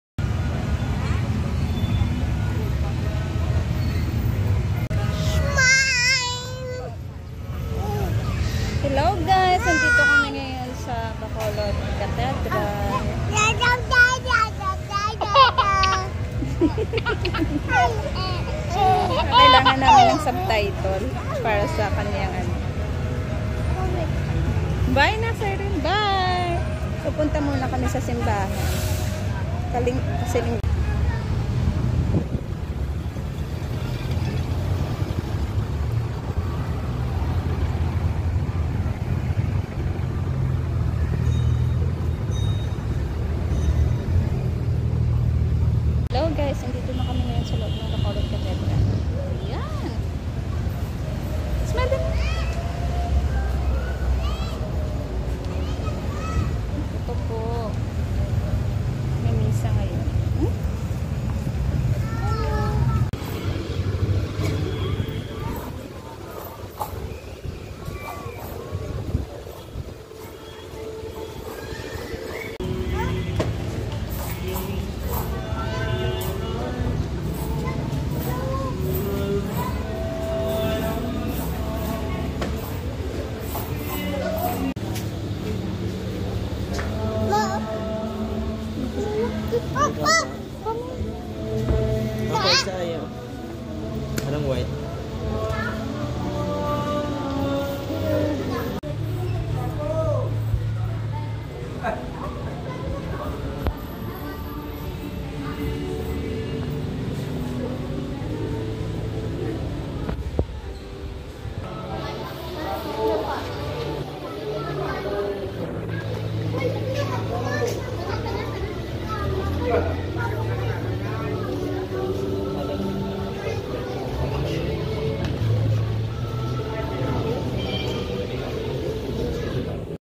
A visit to Bacolod Cathedral Church
Family vlog1 . This is our kind of sunday. Punta muna tayo guys dito sa Bacolod Cathedral to offer our prayers and give thanks to big and small wins in our daily battle sa ating life. Have a blissful and enjoy your week guys.